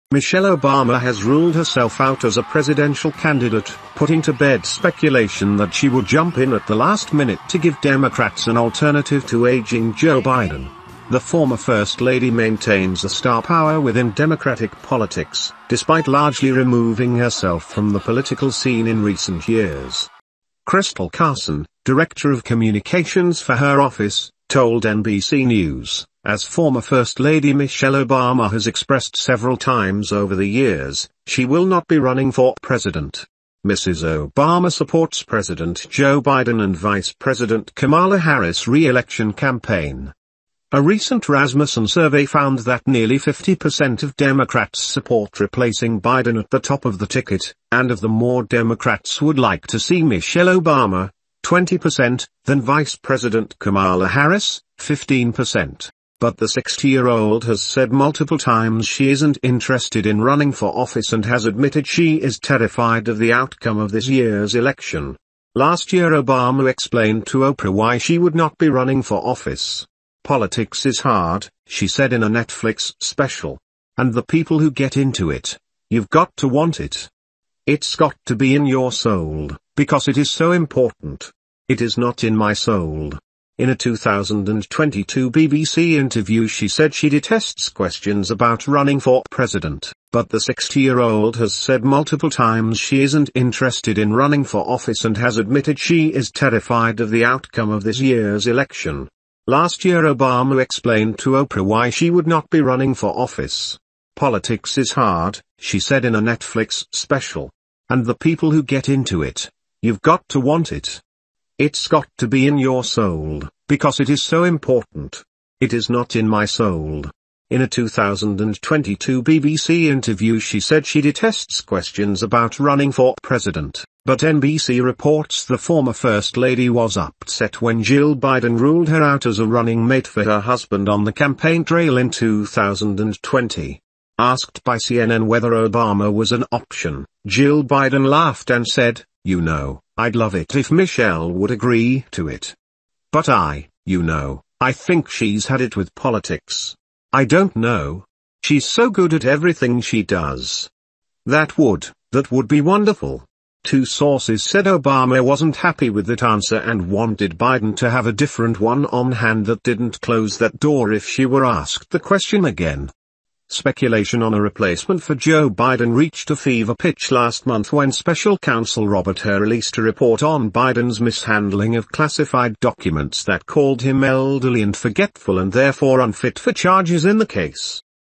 Michelle Obama reveals intentions to run for presidency in 2024 | News54
Michelle Obama, the former First Lady of the United States, has officially stated that she will not be running for president, thereby dispelling speculations suggesting otherwise. This announcement has put an end to the notion that she might enter the presidential race as a last-minute alternative for the Democratic Party, particularly amid concerns about the advancing age of incumbent President Joe Biden.